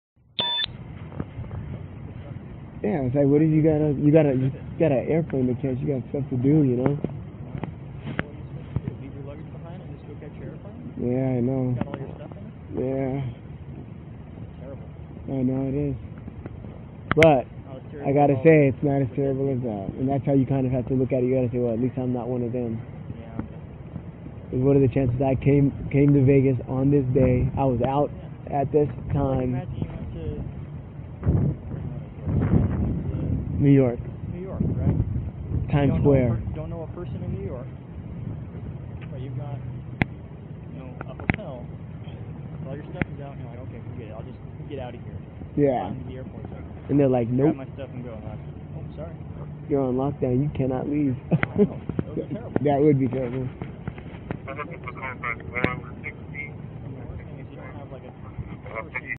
Las Vegas Shooting - LVMPD Release #31 (#00892)
Undetermined officer's footage.

This is body worn camera #892 from LVMPD release #31.

In total, it contains 101 body worn cameras.